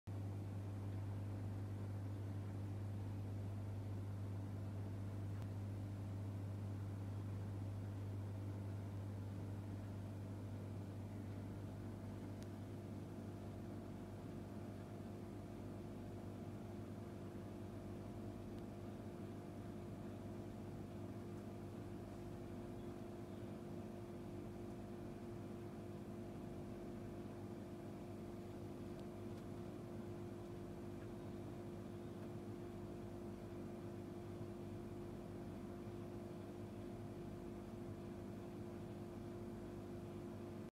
Central American Cichlid’s - Thorichthys Meeki, Thorichthys Maculipinnis & Hypsophrys Nicaraguensis
Thorichthys Meeki (Firemouth Cichlid) Thorichthys Maculipinnis (Ellioti) & Hypsophrys Nicaraguensis (Nicaraguan Cichlid) Central American Cichlid Community. Video taken the day after they were introduced into their 180 gallon, 6’ x 2’ x 2’ tank.